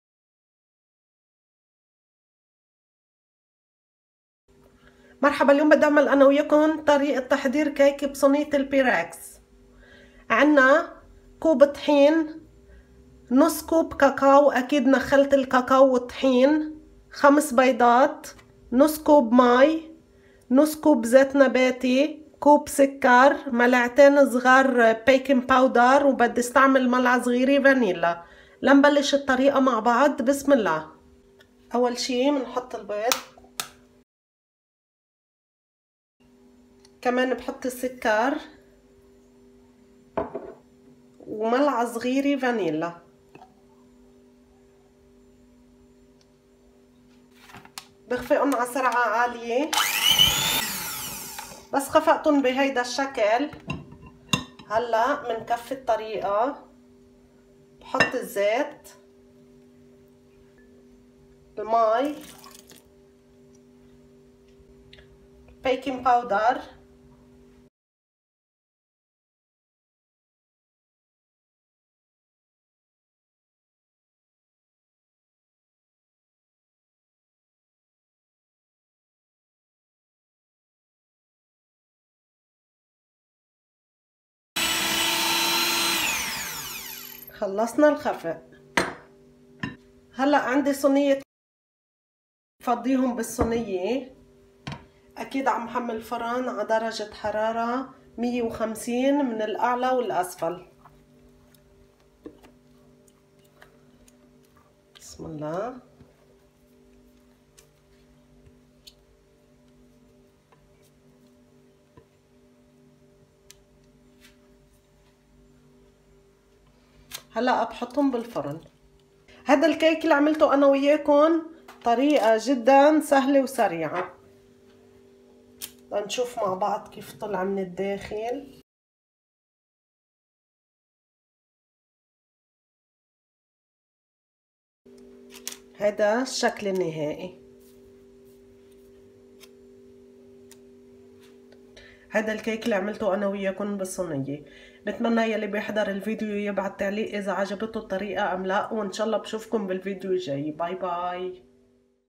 Chocolate Cake - كيك الشوكولا سهل وسريع التحضيربالصنية وبدون حليب
المقادير
٥ بيض
١ كوب سكر
١ ملعقة صغيرة فانيليا
٢ ملعقة صغيرة بيكنبودر
١/٢ كوب زيت
١/٢ كوب ماي
١/٢ كوب كاكاو
١ كوب طحين

Ingredients
5 eggs
1 cup of sugar
1 teaspoon of vanilla
2 teaspoons of baking powder
1/ 2 cup of water
1/2 cup of oil
1 cup of flour
1/2 cup of cocoa powder